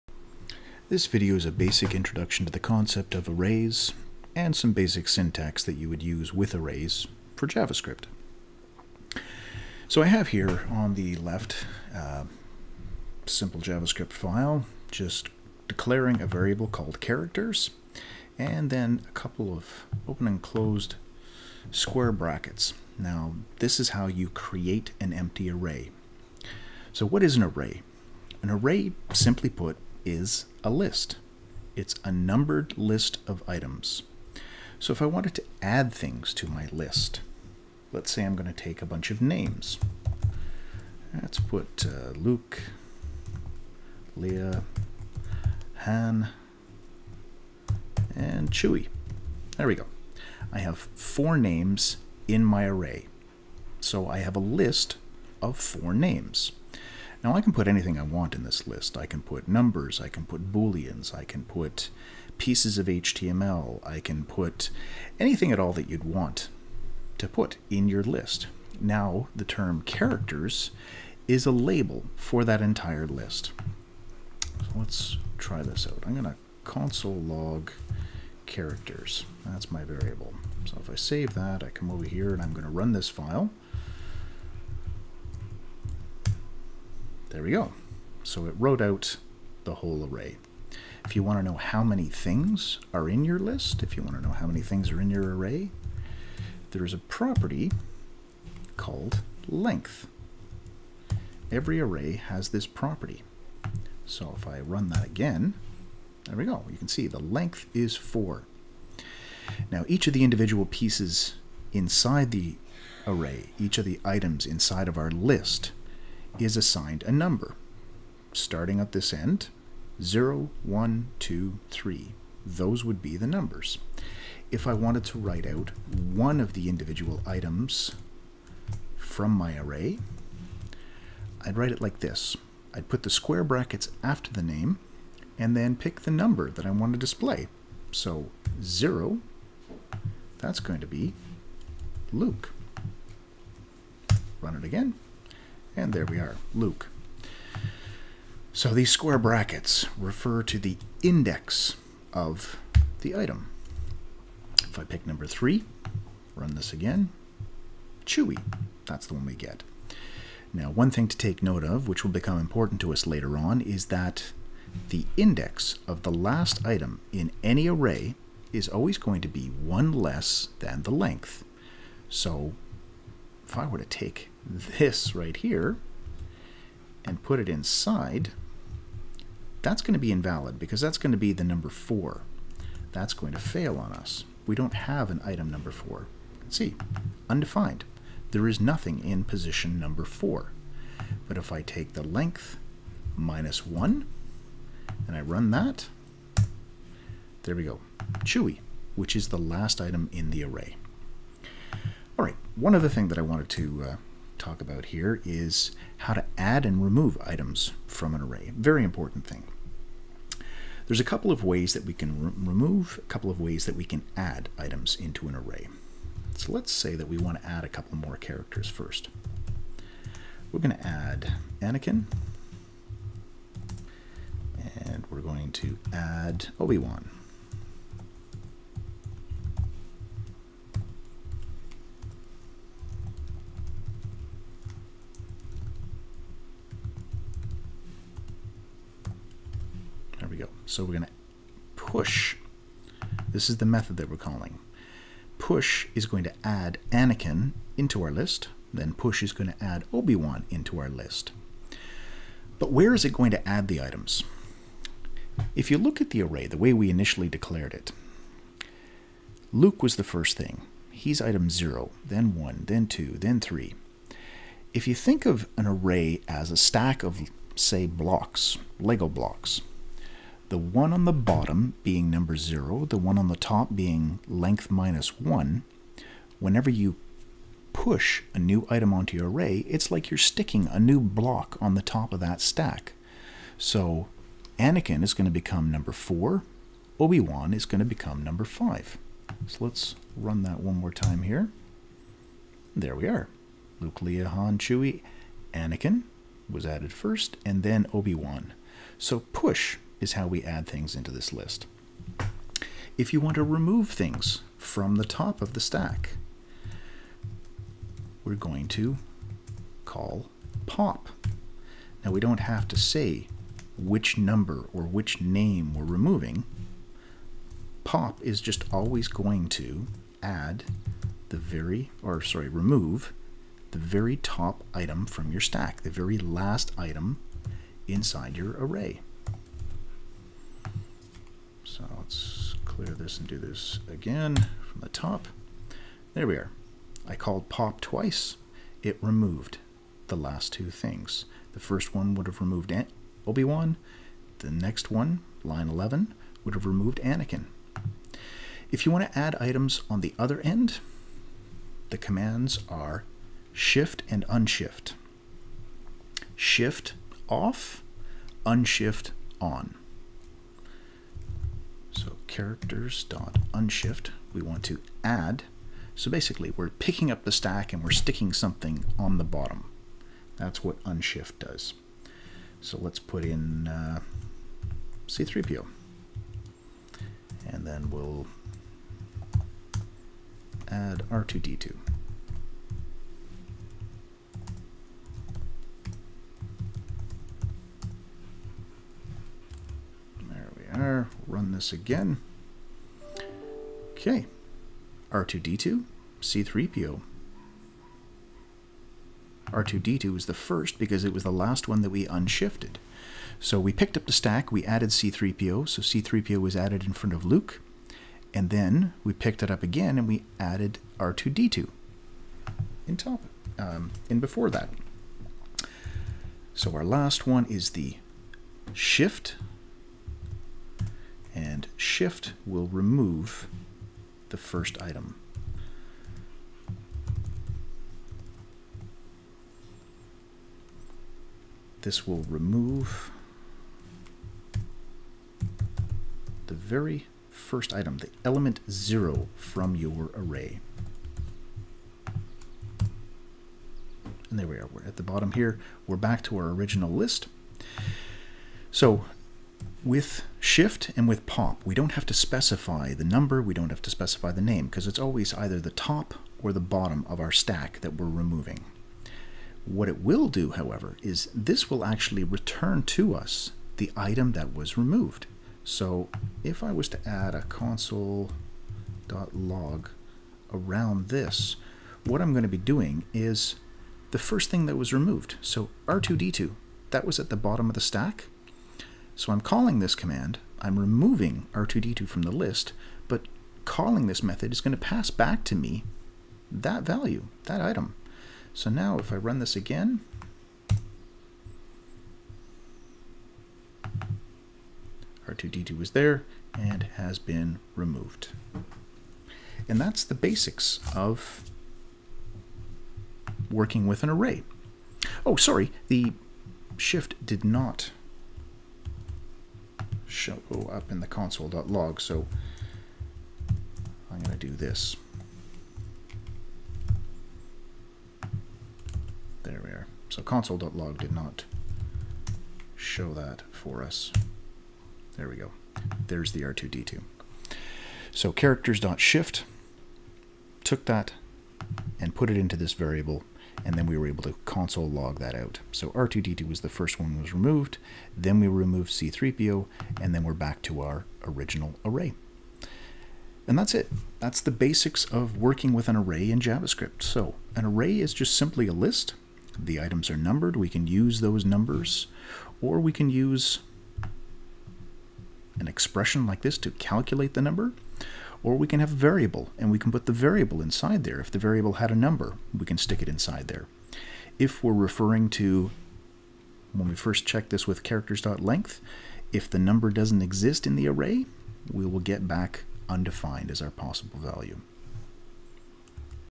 Introduction to Arrays with JavaScript
An introduction to the concept of an Array and the basic syntax for adding and removing items from an Array.